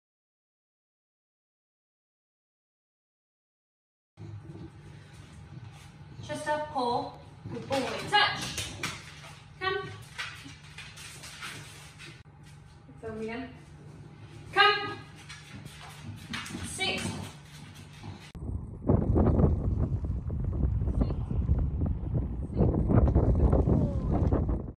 Dog Training: Chester (English Bulldog) - Paw, Touch with nose, Stand between legs, Come, Sit
Showreel for Chester the English Bulldog model
Skills include:
- Paw
- Touch with nose
- Stand between legs
- Come
- Sit